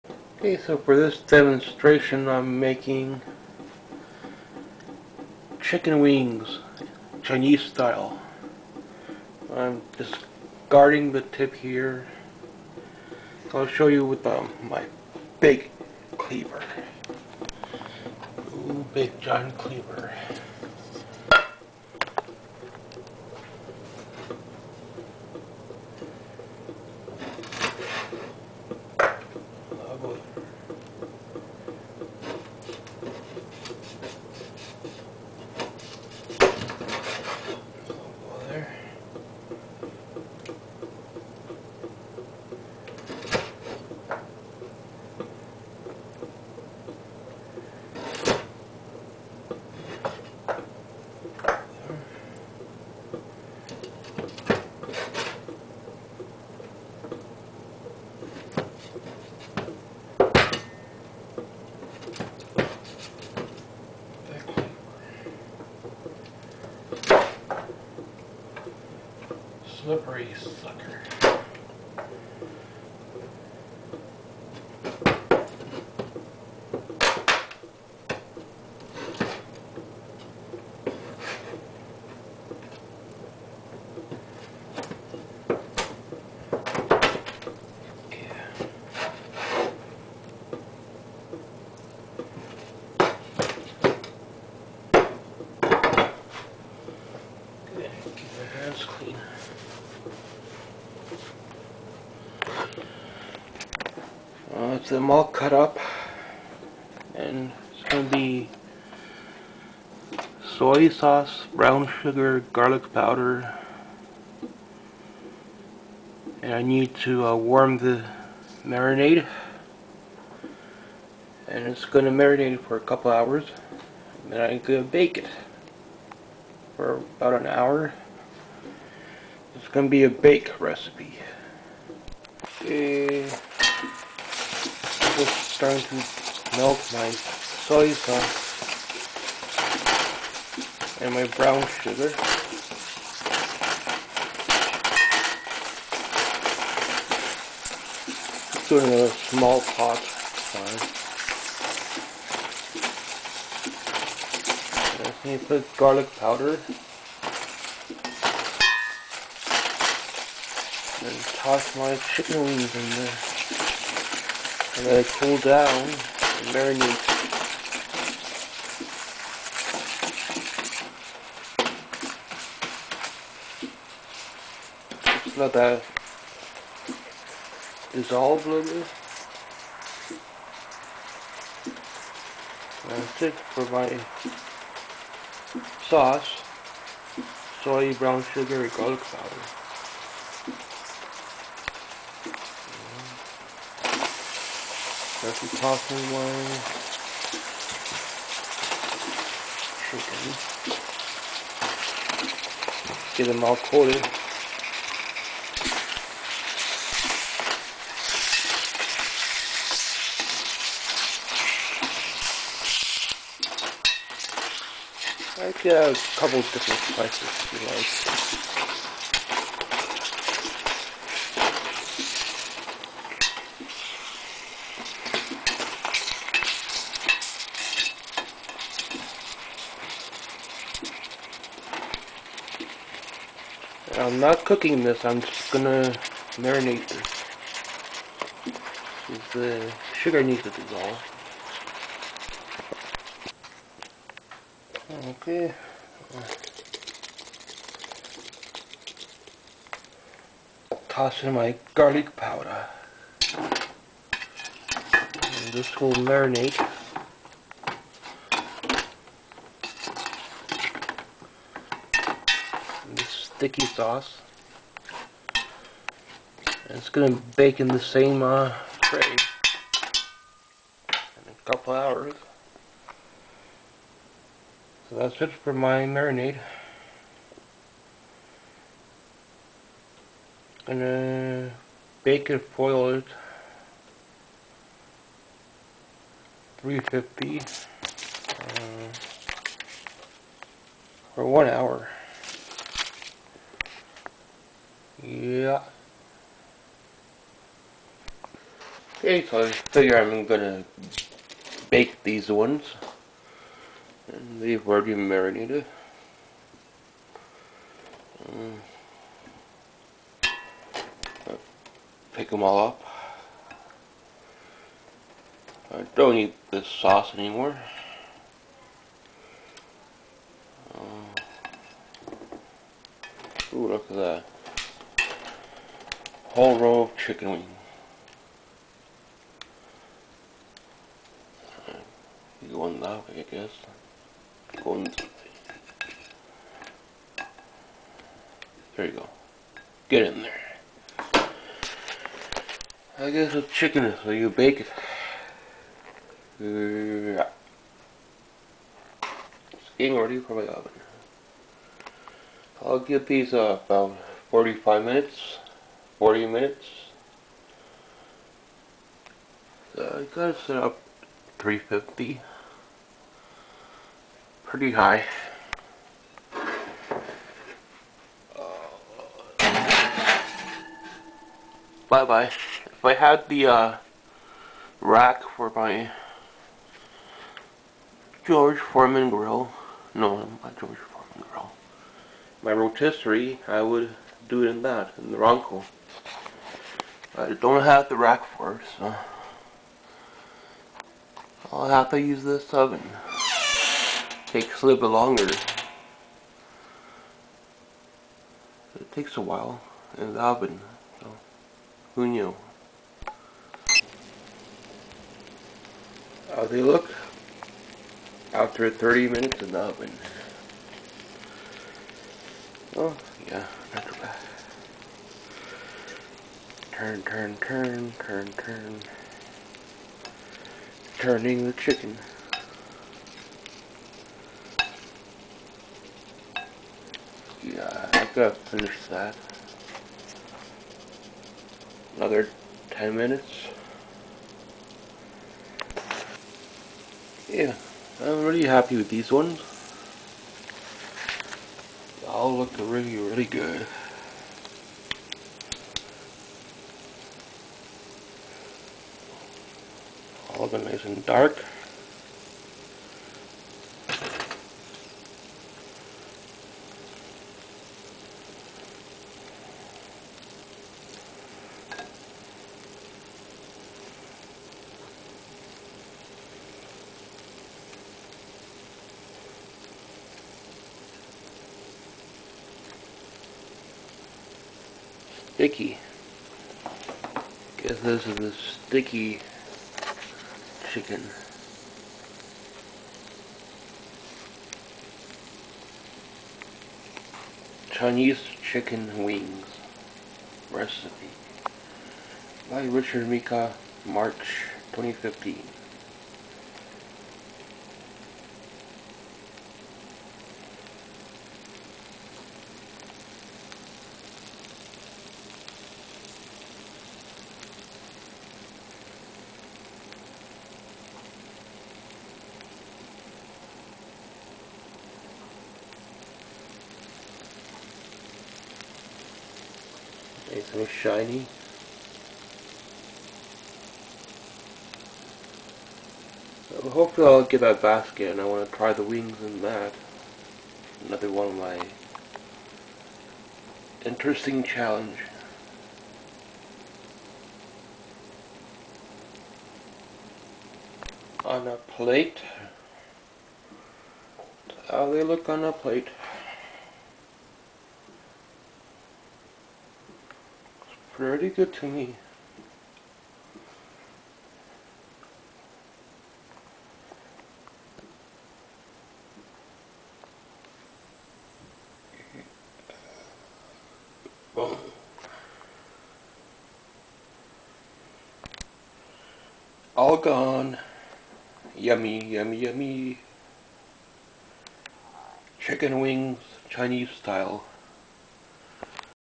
chinese sticky chicken wings
marinade

1 tblsp chinese rice wine* option
2 tblsp Maggi sauce  or light soy sauce
2 tblsp  sugar (brown) *
2 tsp ginger powder
2 tsp sesame oil
i seeded chopped chilli (optional) heat or franks red hot
Handful of fresh chopped coriander / cilantro   at end garnish
sesame seeds  garnish option

dissolve over heat add meat of choice wings  chicken pices

marinated few hour bake as in tray about 1 hour turning 350